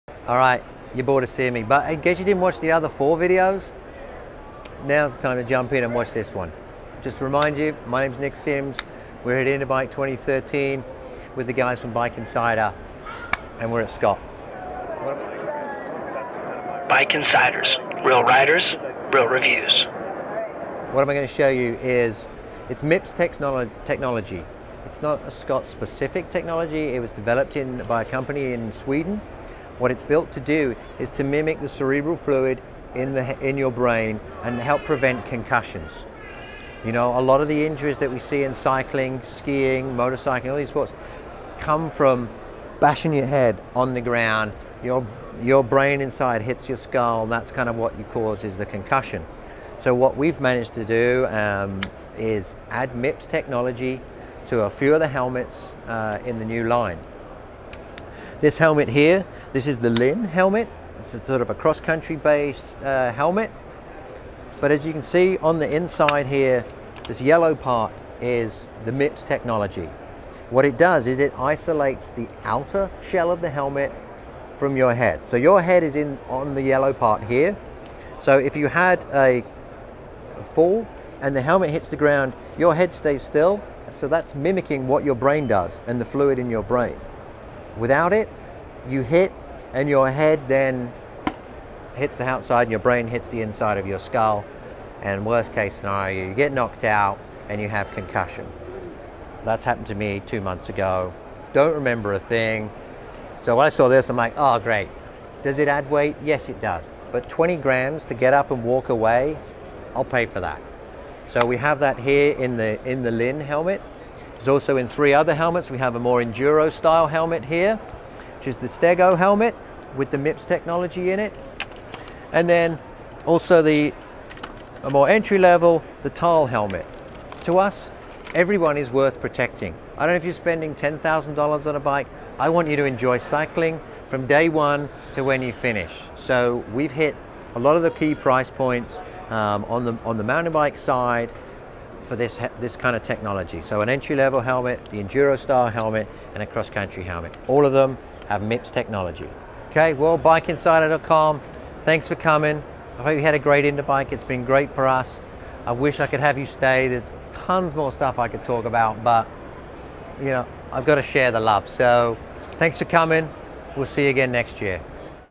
Scott Lin 2014 Helmets - Bike Insiders - Mips Helmets 2013 Interbike
At Interbike 2013 Scott introduces MIPS technology to their helmet line. The brain is surrounded by a low-friction cushion of cerebrospinal fluid. MIPS imitates the brain's way of protecting itself by giving the helmet its own low-friction layer between the outer shell and the liner, to absorb much of the energy created by an oblique blow to the head.